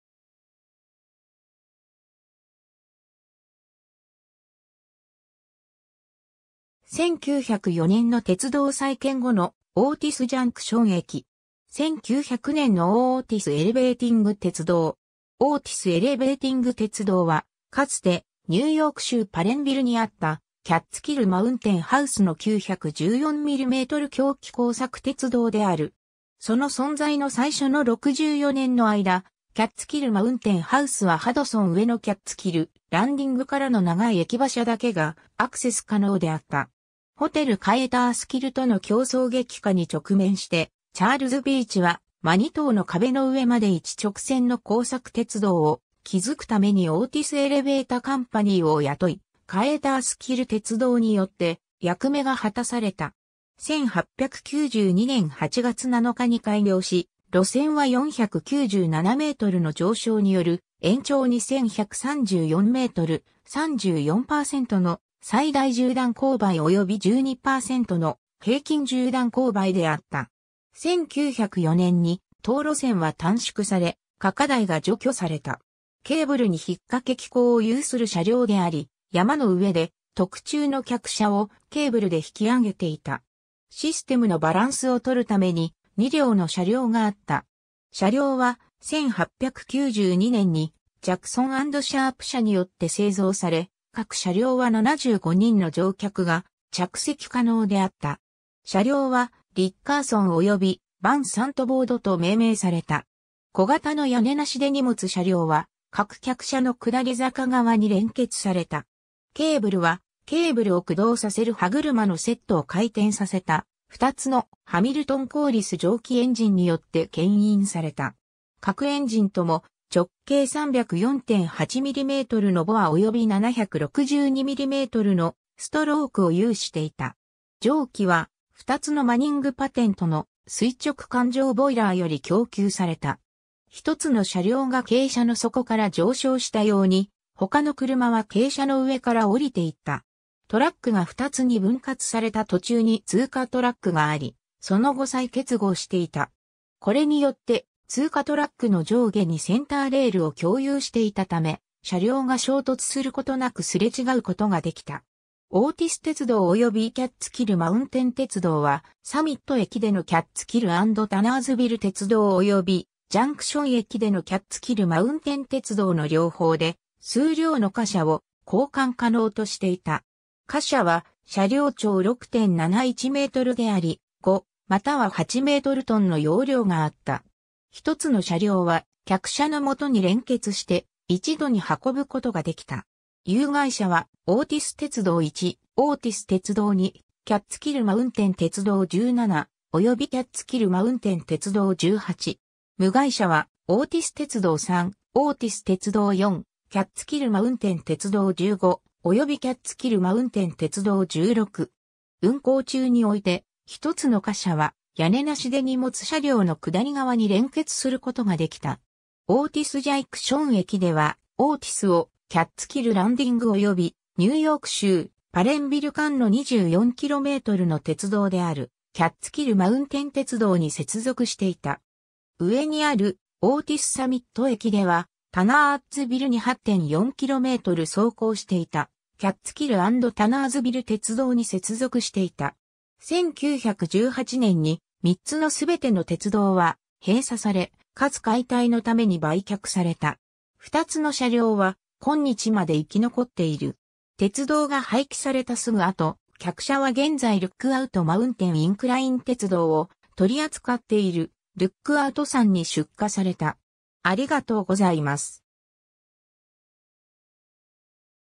オーティス・エレベーティング鉄道
オーチス・エレベータ・カンパニー
1904年の鉄道再建後のオーティス・ジャンクション駅。
 1900年のオーティス・エレベーティング鉄道。
 オーティス・エレベーティング鉄道（オーティスエレベーティングてつどう、英: Otis Elevating Railway）は、かつてニューヨーク州パレンビルにあったキャッツキル・マウンテン・ハウスの914mm (3ft)狭軌鋼索鉄道である。
 その存在の最初の64年の間、キャッツキル・マウンテン・ハウスはハドソン上のキャッツキル・ランディングからの長い駅馬車だけがアクセス可能であった。
 ホテル・カエータースキルとの競争激化に直面して、チャールズ・ビーチはマニトウの壁の上まで一直線の鋼索鉄道を築くためにオーティス・エレベータ・カンパニーを雇い、カエータースキル鉄道によって役目が果たされた。
 1892年8月7日に開業し、路線は497m (1,630ft)の上昇による延長2,134m (7,000ft)、34%の最大縦断勾配および12%の平均縦断勾配であった。
 1904年に、当路線は短縮され、下架台が除去された。
 ケーブルに引っ掛け機構を有する車両であり、山の上で特注の客車をケーブルで引き上げていた。
 システムのバランスをとるために、2両の車両があった。
 車両は、1892年にジャクソン・アンド・シャープ社によって製造され、各車両は75人の乗客が着席可能であった。
 車両は、「リッカーソン (Rickerson) 」および「ヴァン・サントヴォード (Van Santvoord) 」と命名された。
 小型の屋根なし手荷物車両は、各客車の下り坂側に連結された。
 ケーブルは、ケーブルを駆動させる歯車のセットを回転させた、2つのハミルトン・コーリス蒸気エンジンによって牽引された。
 各エンジンとも、直径304.8mm (12in) のボアおよび762mm (30in)のストロークを有していた。
 蒸気は、2つのマニング・パテントの垂直管状ボイラーより供給された。
 1つの車両が傾斜の底から上昇したように、他の車は傾斜の上から降りていった。
 トラックが2つに分割された途中に通過トラックがあり、その後再結合していた。
 これによって、通過トラックの上下にセンターレールを共有していたため、車両が衝突することなくすれ違うことができた。
 オーティス鉄道およびキャッツキル・マウンテン鉄道は、サミット駅でのキャッツキル・アンド・タナーズビル鉄道およびジャンクション駅でのキャッツキル・マウンテン鉄道の両方で、数両の貨車を交換可能としていた。
 貨車は、車両長6.71m (22ft)であり、5または8米トン（4.46または7.14英トン、4.54または7.26t）の容量があった。
 1つの車両は、客車の下に連結して、一度に運ぶことができた。
 有蓋車は、オーティス鉄道1、オーティス鉄道2、キャッツキル・マウンテン鉄道17、およびキャッツキル・マウンテン鉄道18。
 無蓋車は、オーティス鉄道3、オーティス鉄道4、キャッツキル・マウンテン鉄道15、およびキャッツキル・マウンテン鉄道16。
 運行中において、1つの貨車は屋根なし手荷物車両の下り側に連結することができた。
 オーティス・ジャンクション駅（右上の1904年再建直後の写真）では、オーティスをキャッツキル・ランディングおよびニューヨーク州パレンビル間の24km (15mi)の鉄道である、キャッツキル・マウンテン鉄道に接続していた。
 上にあるオーティス・サミット駅では、タナーズビルに8.4km (5.2mi)走行していた、キャッツキル・アンド・タナーズビル鉄道に接続していた。
 1918年に、3つのすべての鉄道は閉鎖され、かつ解体のために売却された。
 2つの車両は、今日まで生き残っている。
 鉄道が廃棄されたすぐ後、客車は現在ルックアウト・マウンテン・インクライン鉄道を取り扱っている、ルックアウト山に出荷された。